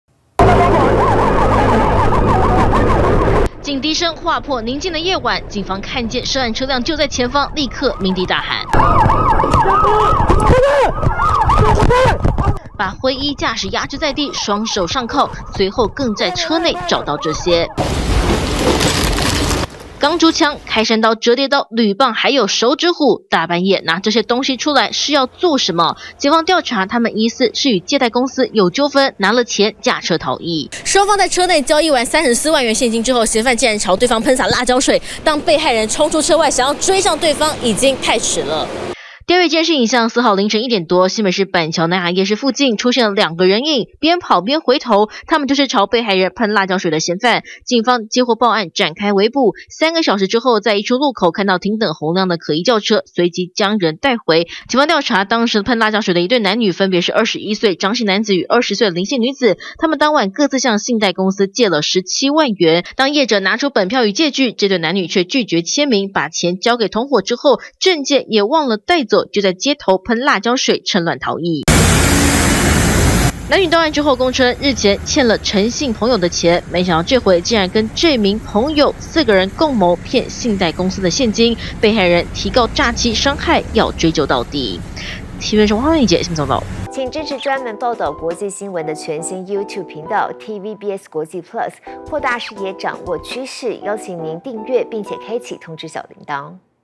誆借34萬拒簽本票!男女各拿17萬 邊跑邊噴辣椒水｜TVBS新聞
新北市一對男女被指控，與信貸公司相約南雅夜市，借貸現金，沒想到車內錢拿出來，他們卻不肯簽借據，一走出車外，就噴辣椒水，警方獲報到場，三小時內逮捕所有共犯，更在涉案車輛，找到刀械與鋼珠槍！